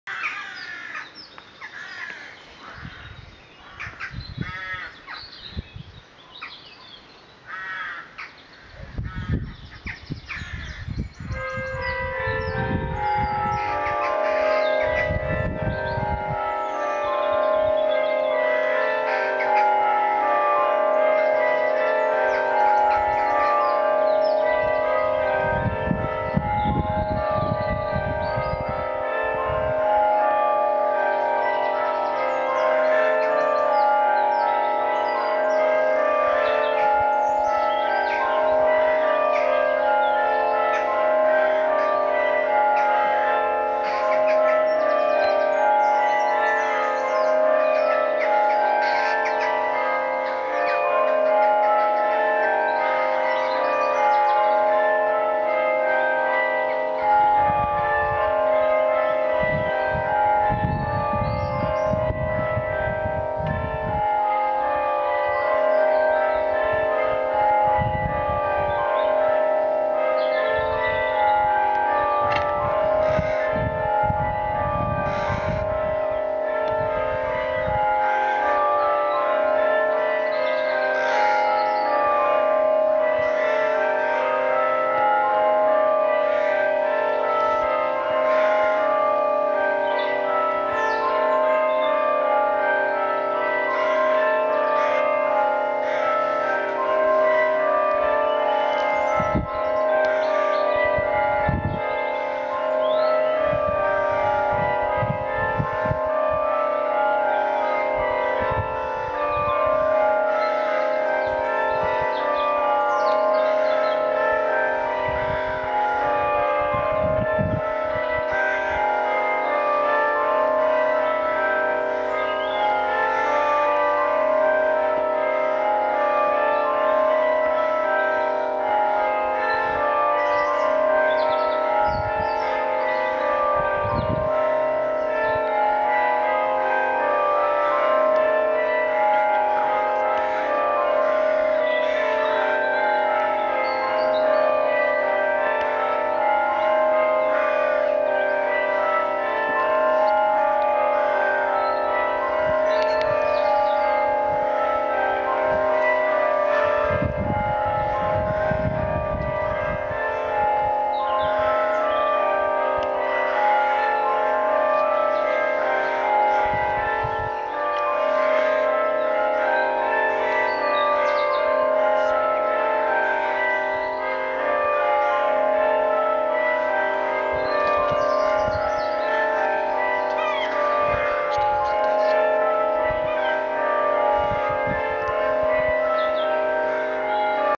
Chelveston-St-John-Baptist-Bell-Carillon-20-May-2012.MOV
The Parish Church of St John the Baptist Chelveston-cum-Caldecott, Northamptonshire has installed a carillon as a replacement for its bells which are no longer serviceable.  The carillon sounds automatically at 08:47 on a Sunday before the 09:00 service.  The peal is followed by a tolling bell 5 minutes before the service.